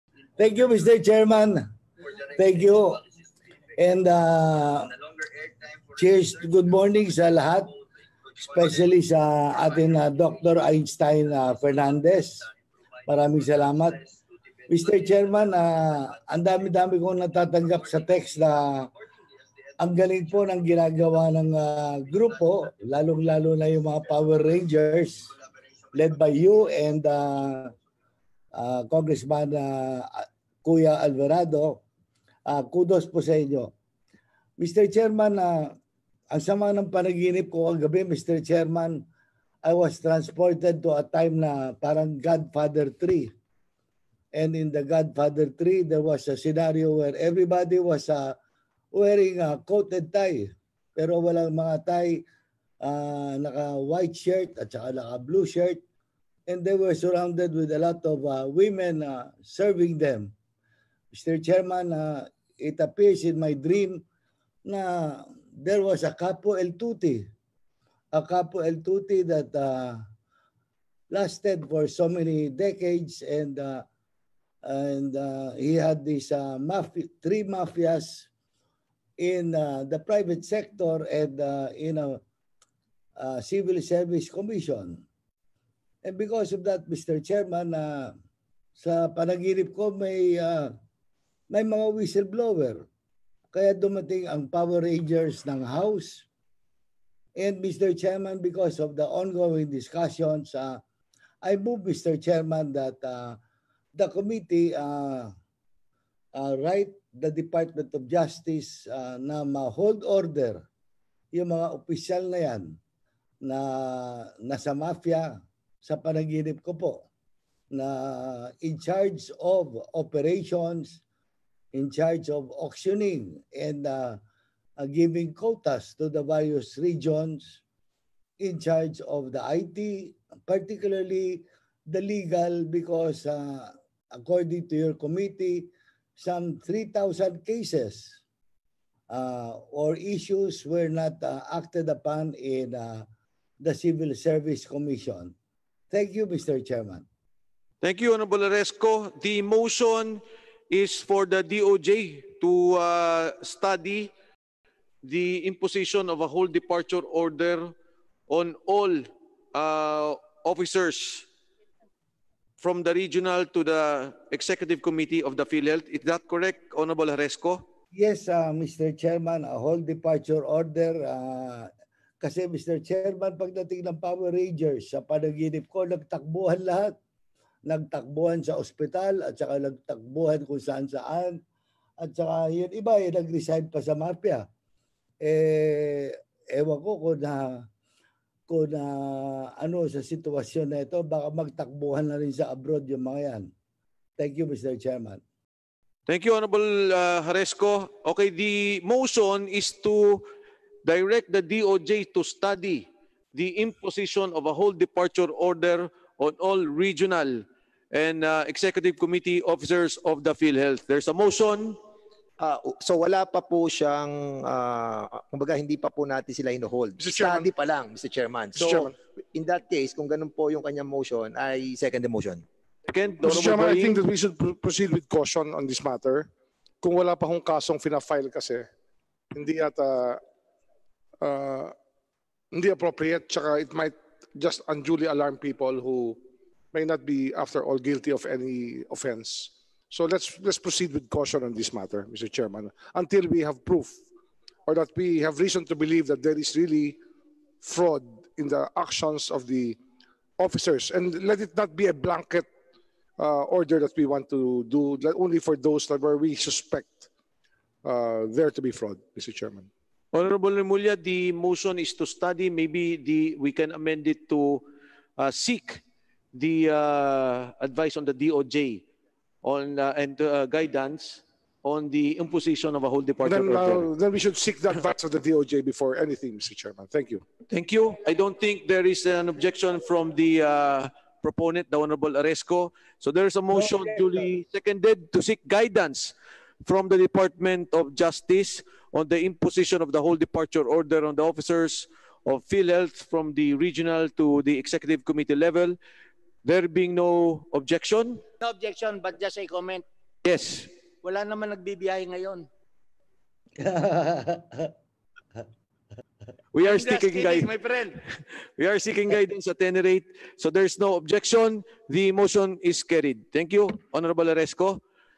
Bizarre ‘GodFather’ dream makes lawmaker seek hold departure orders vs PhilHealth officials
More videos on Rappler The Godfather meets the Power Rangers in Aklan congressman Teodorico Haresco Jr's dream